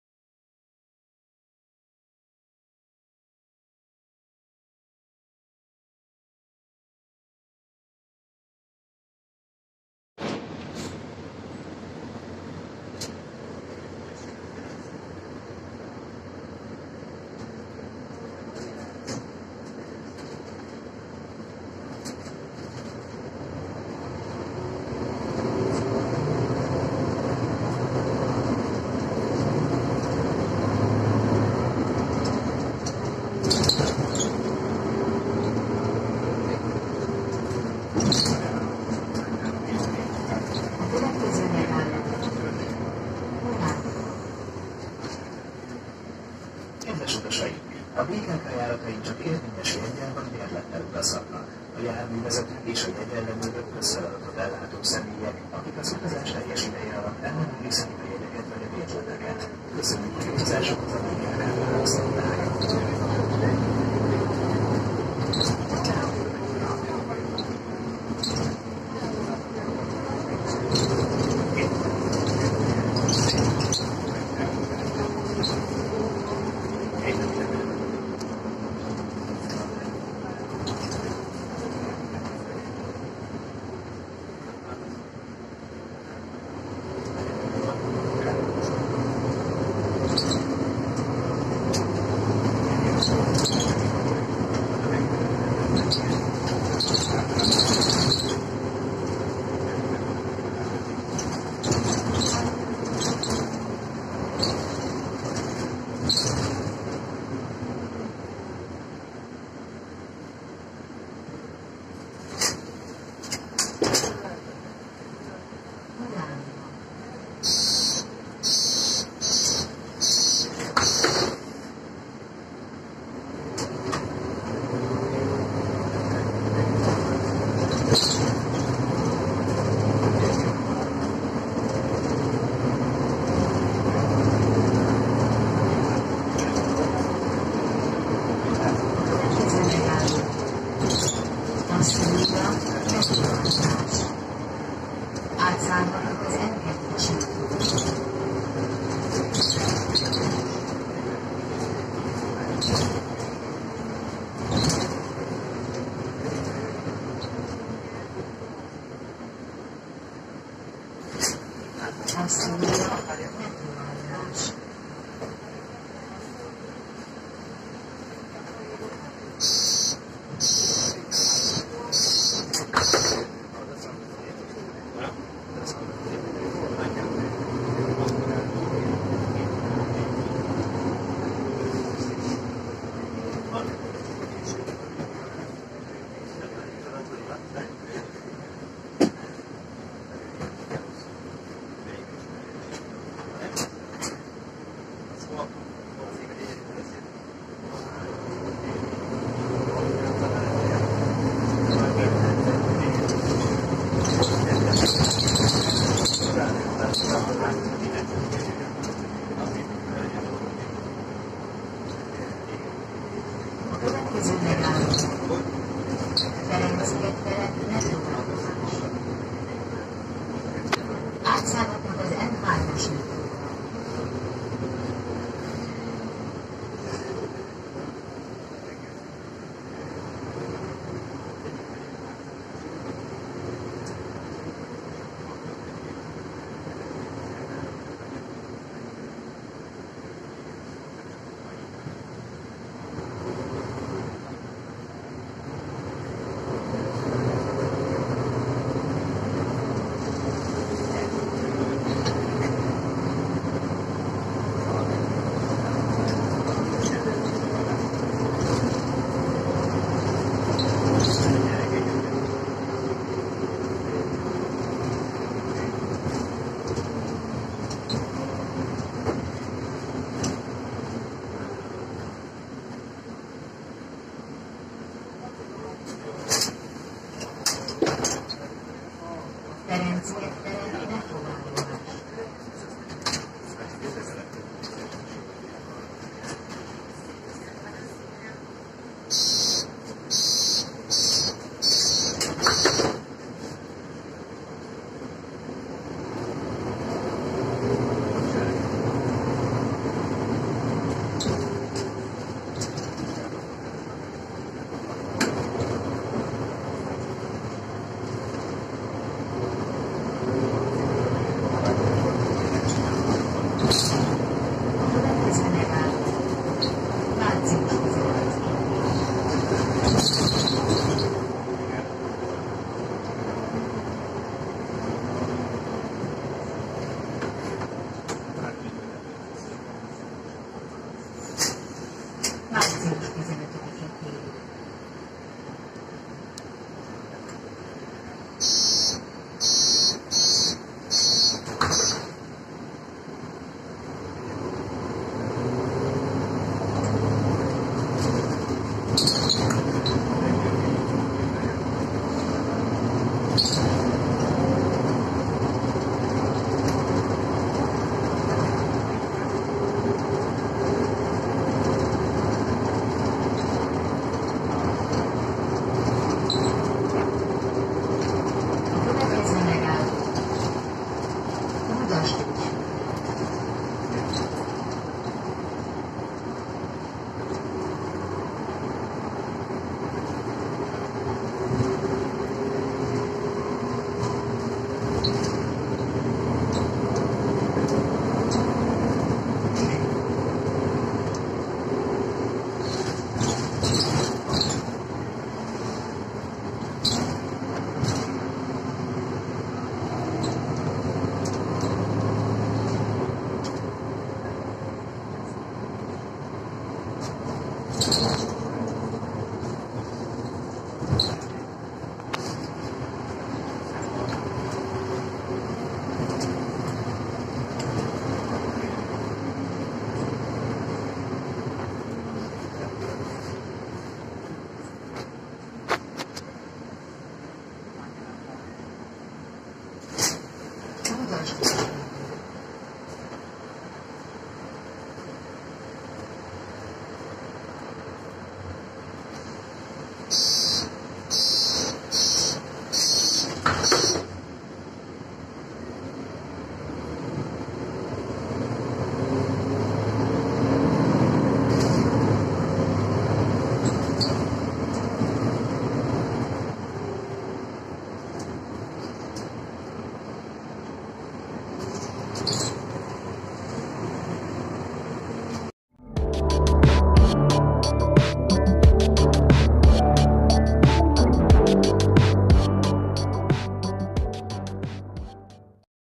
Budapest busz Mercedes Conecto G Next Gen (AA-DR-761)
Első utazásom egy AA-DR-rel. Mivel a 7-es busz mögfötte volt, ezért most rendesen ki volt használva.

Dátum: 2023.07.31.
Típus: Mercedes Conecto G Next Gen
Rendszám: AA-DR-761
Járat: 107
Tulajdonos: BKV
Telephely: Kőbánya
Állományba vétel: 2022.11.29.

📱 A videók Xiaomi Redmi 7-tel készülnek.